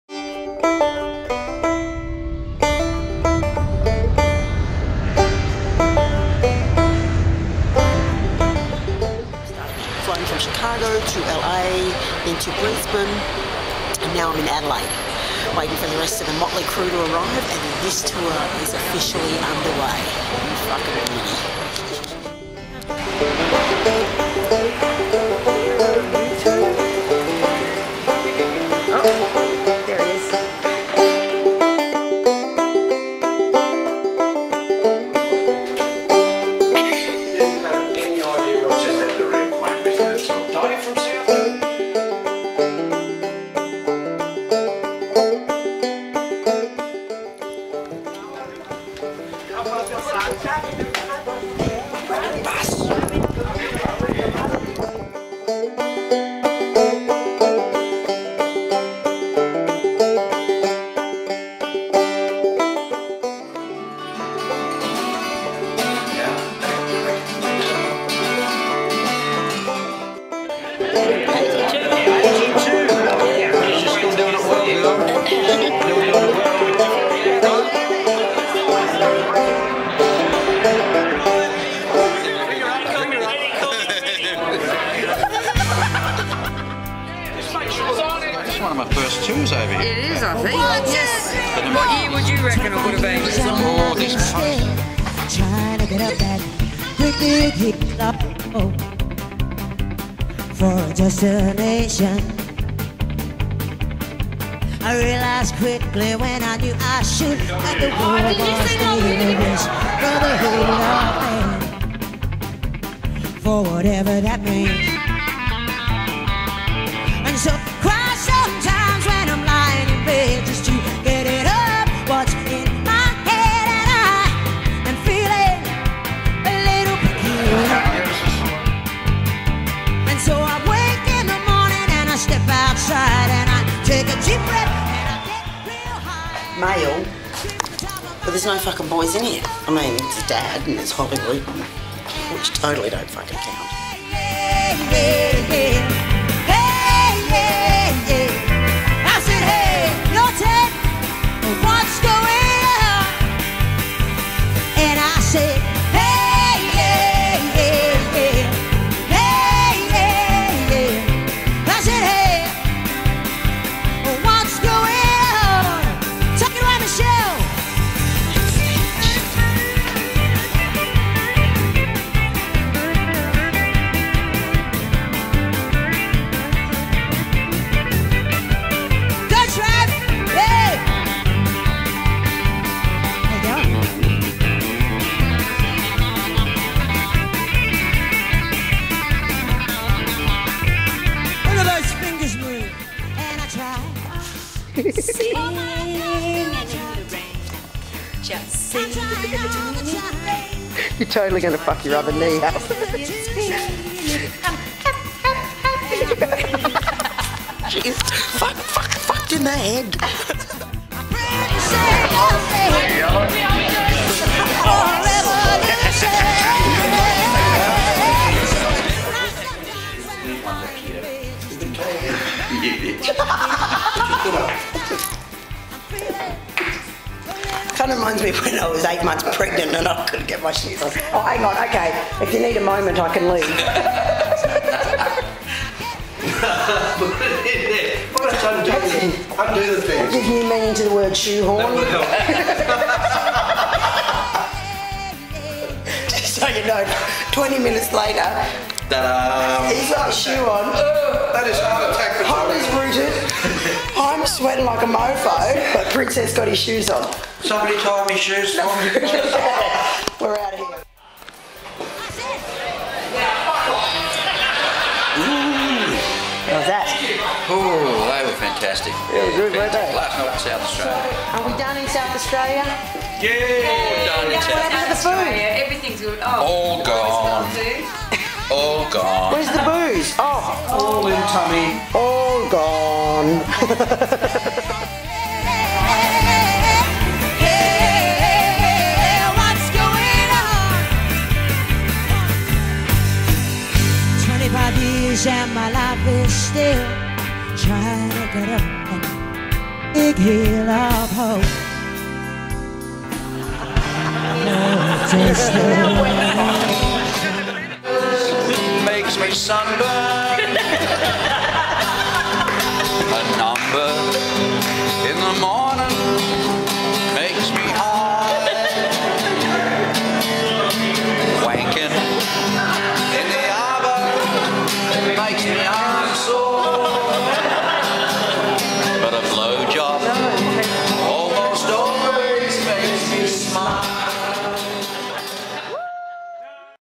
South Australia Tour 2010 VLOG
Jenny Talia on tour as part of the Kevin Bloody Wilson EXCESS ALL AREAS World Tour - the South Australian leg